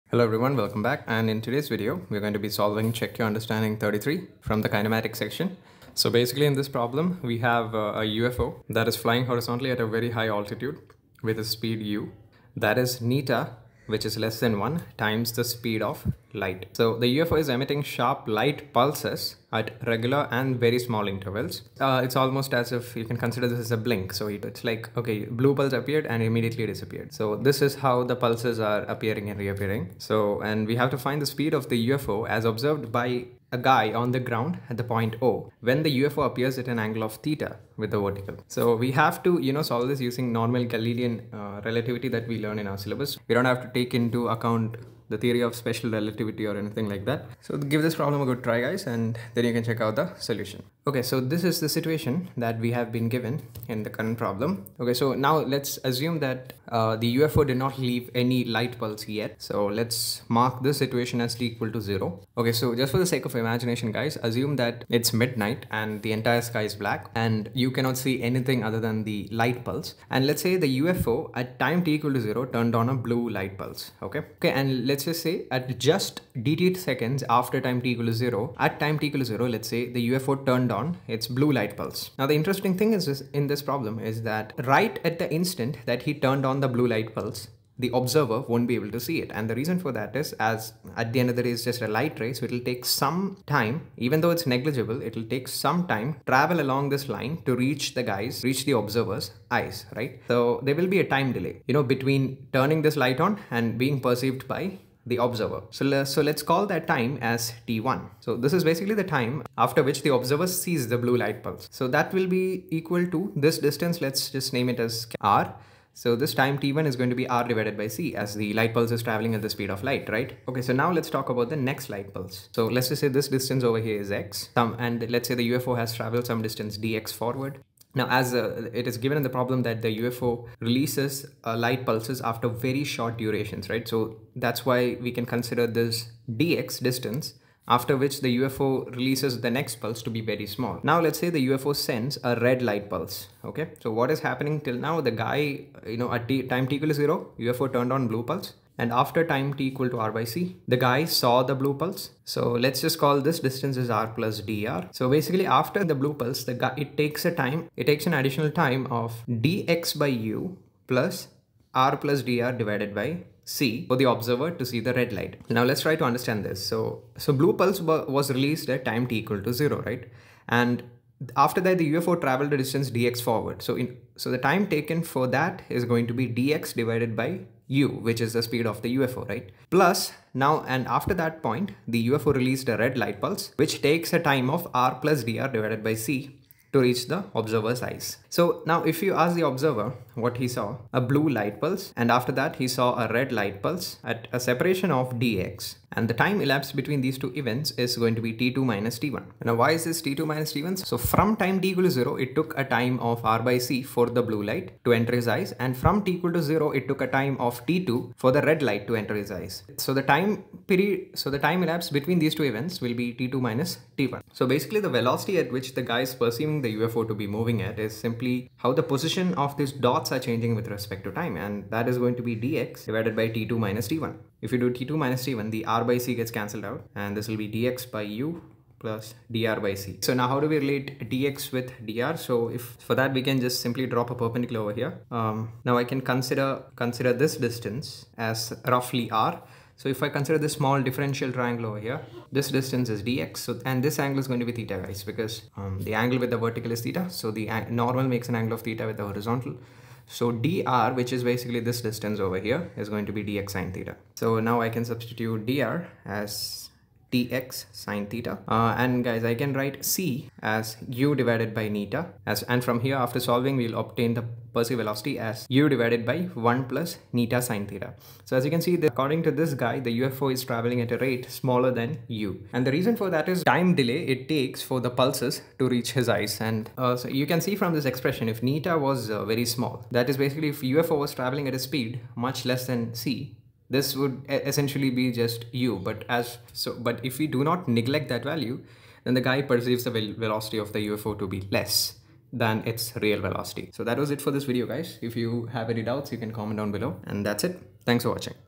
[Jee Adv] Perceived Velocity Of A UFO By An Observer On Ground | CYU 33 | Kinematics | Pathfinder
Pathfinder CYU 33 Kinematics
In a particular scene of a science fiction movie, a UFO is flying horizontally at a very high altitude with a speed that is n times of speed
 of light. The UFO is emitting sharp light pulses at regular and very small intervals. Find the speed of the UFO recorded by an observer on the ground at point when the UFO appears at an angle with the vertical. Ignore relativistic corrections. A UFO (un-identified flying object) is believed to be a space ships used by aliens.